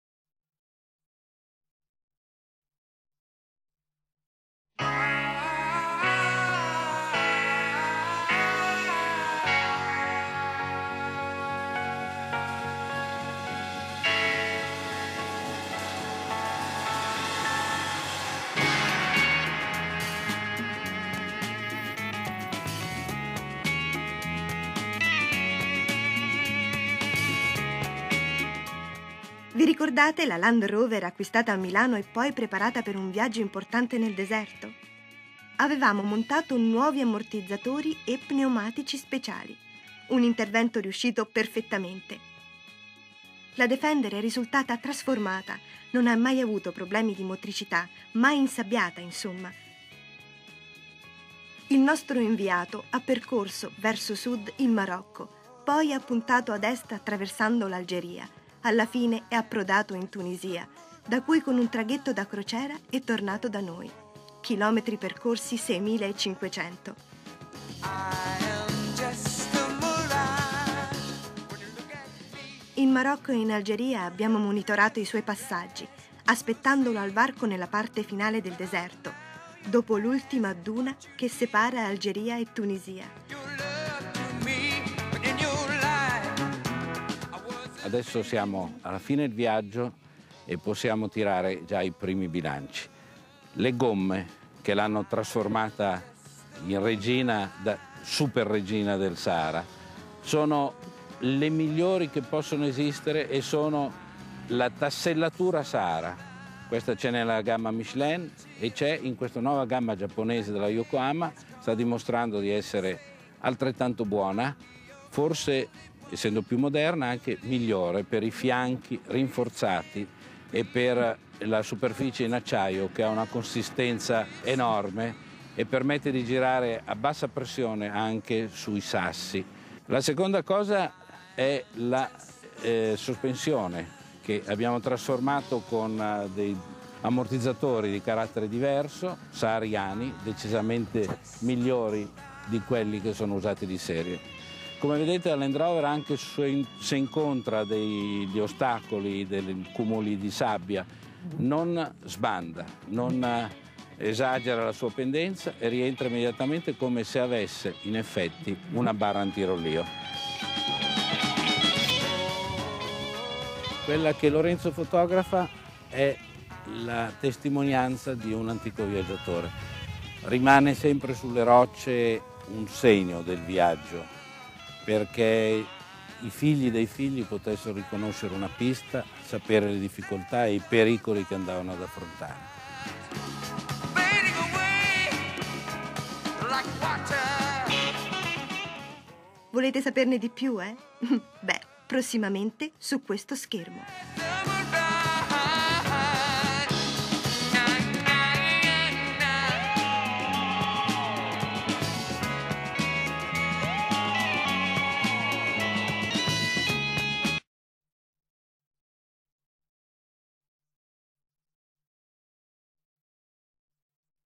yokoama road test gomme sahara
provare per credere, le sahara di Yokoama hanno battuto le Michelin S !!!